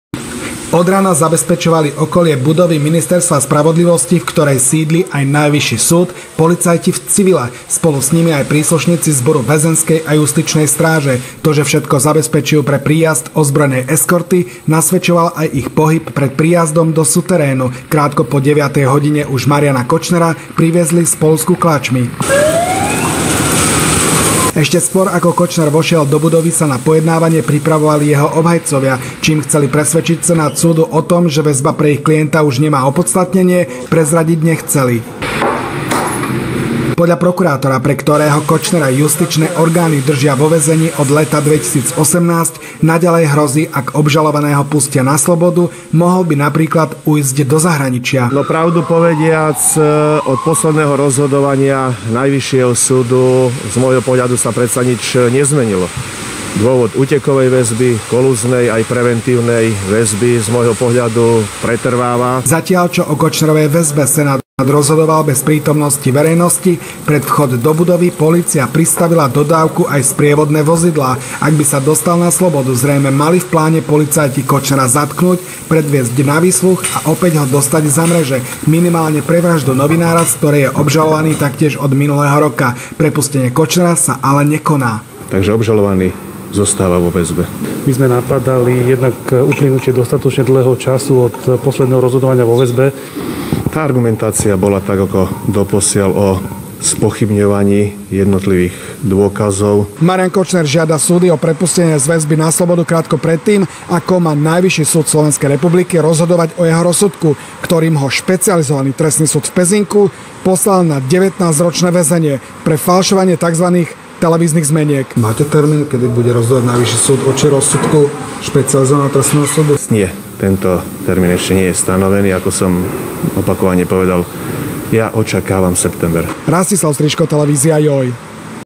Najvyšší súd rozhodol že Marián Kočner ostáva vo väzbe
Marian Kočner, ktorý je už druhý rok vo väzení pre falšovanie zmeniek, sa snaží dostať na slobodu. Dopoludnia o jeho ďalšej žiadosti rozhodoval Najvyšší súd Slovenskej republiky. A hoci išlo o neverejné rozhodovanie súdu, Kočnera na pojednávanie priviezla do centra Bratislavy ozbrojená eskorta. Jeho advokáti chceli uviesť dôvody, prečo by sa mal ich klient dostať z väzby, osobne až pred sudcami
ZDROJ: Noviny TV JOJ